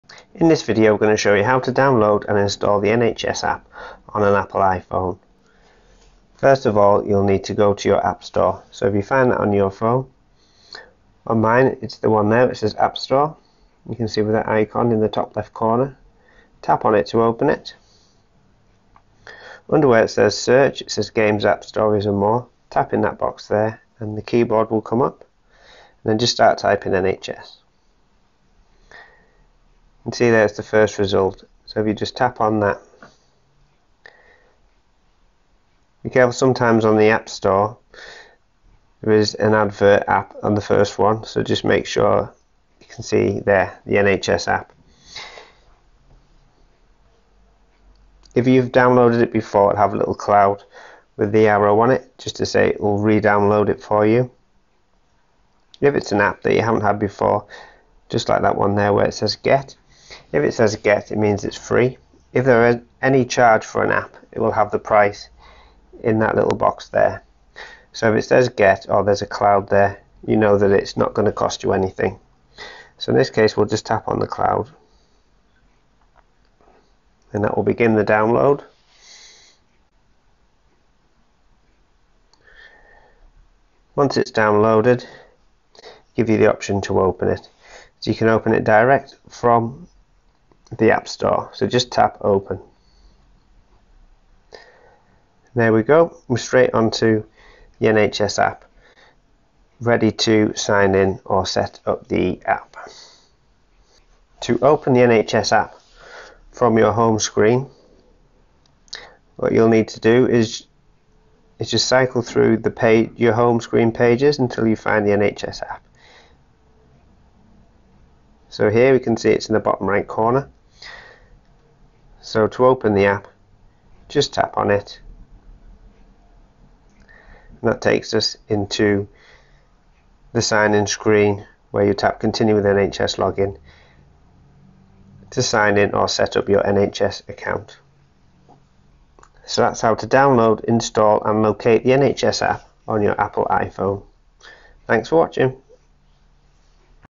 NHS App download and install on iPhone
This tutorial will show you how to download and install the NHS app on an Apple iPhone